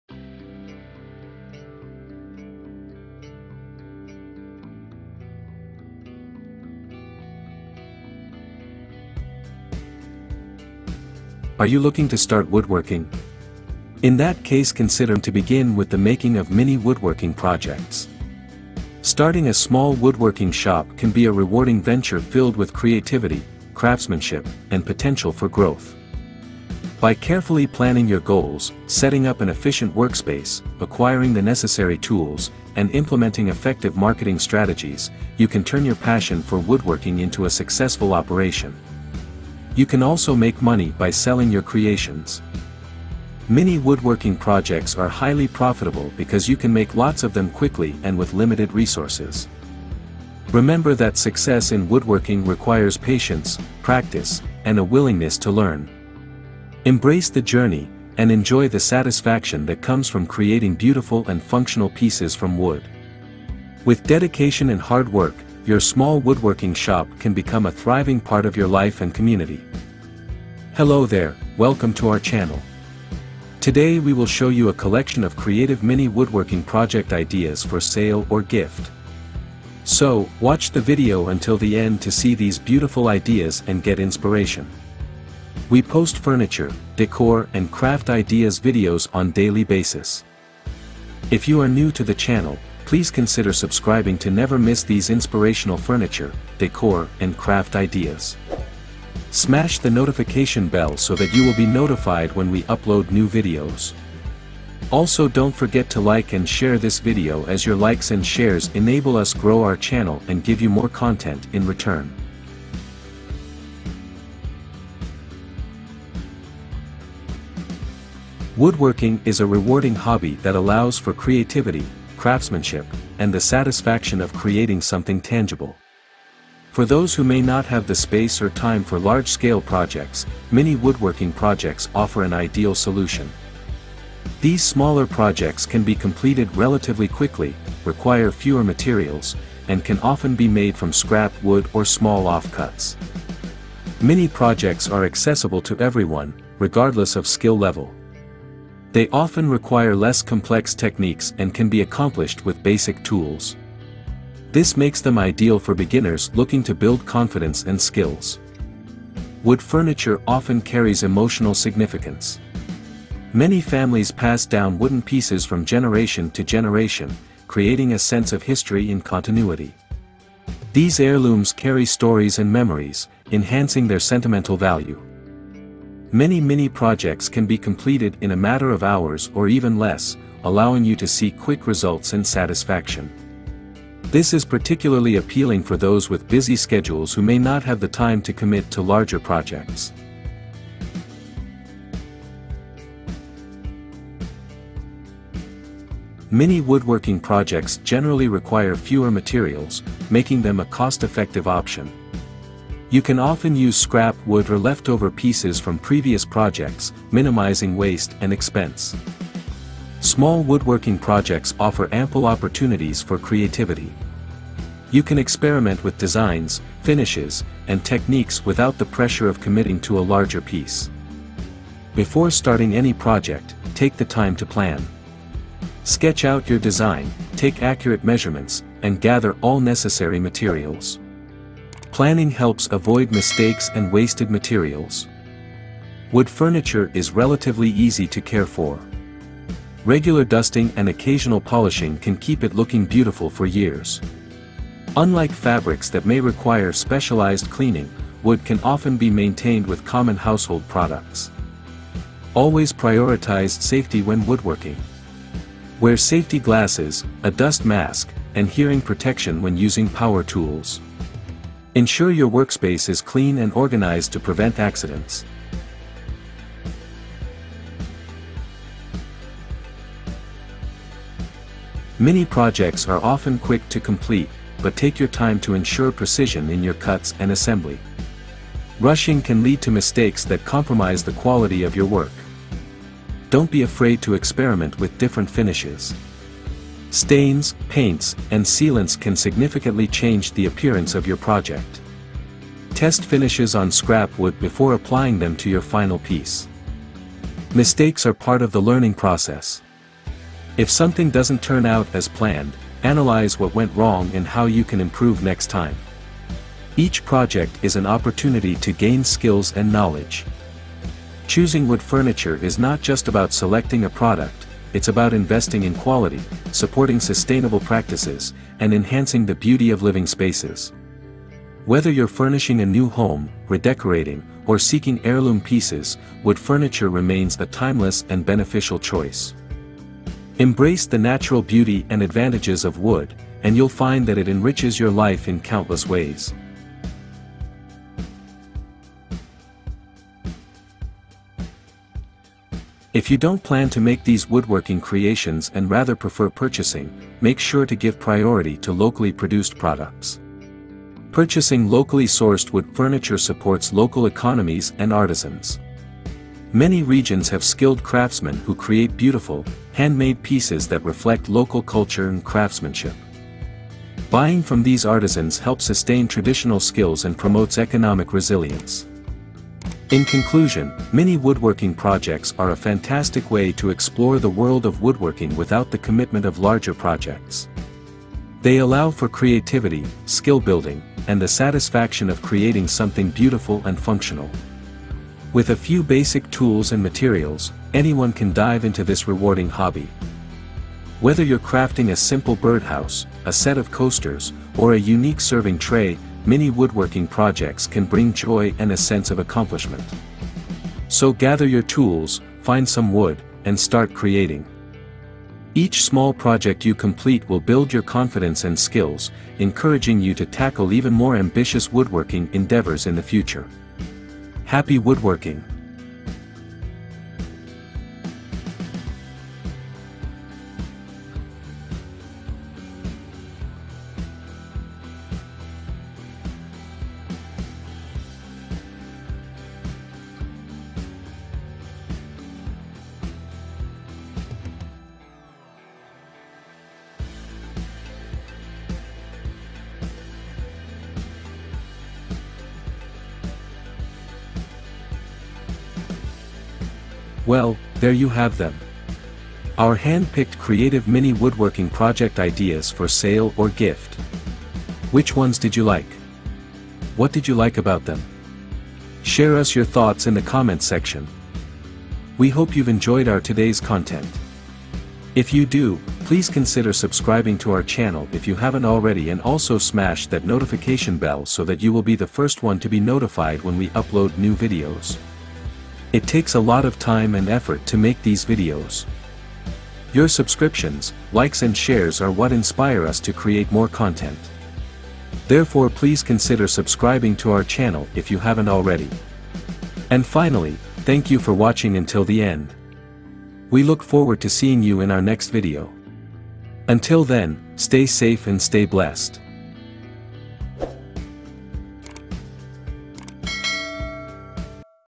Make money with these creative mini woodworking project ideas / mini woodworking projects for profit
Today we will show you a collection of Make money with these creative mini woodworking project ideas / mini woodworking projects for profit. Watch these useful wood furniture ideas and some wooden decorative pieces ideas. If you watch the video until the end you will see tons beautiful ideas and get inspiration.